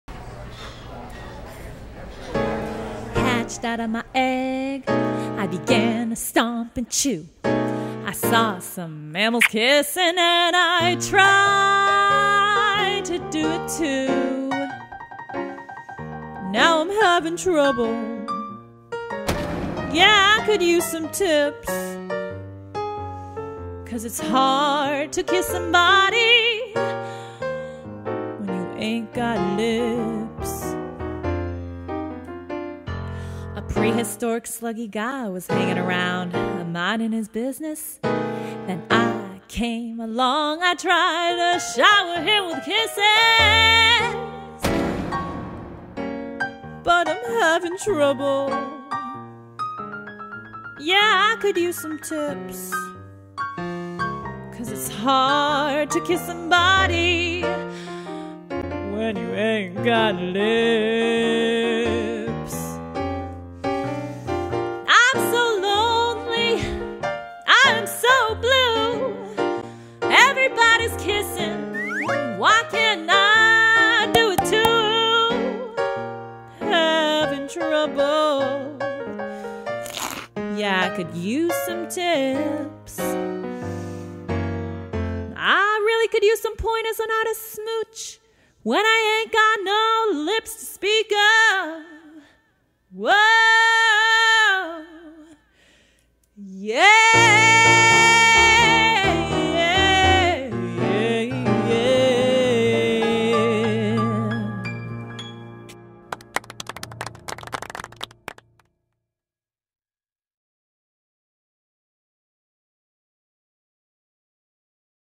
Dinosaur Kisses by David Ezra Stein - Book Trailer
Dinosaur Kisses by David Ezra Stein

WHOMP! An energetic young dinosaur figures out her own way to give a kiss in the latest from the creator of the Caldecott Honor-winning Interrupting Chicken.

For newly hatched dinosaur Dinah, the world is an exciting place. There is so much to see and do. She tries this -- STOMP! And she tries that --CHOMP! Then she sees a kiss and knows just what she wants to try next. Who can she kiss? And after a few disastrous attempts, can she figure out HOW to give someone a kiss without whomping, chomping, or stomping them first? Young children will chuckle and cheer when Dinah finds just the right creature for her dinosaur kisses in this funny new picture book from David Ezra Stein.

On Sale August 2013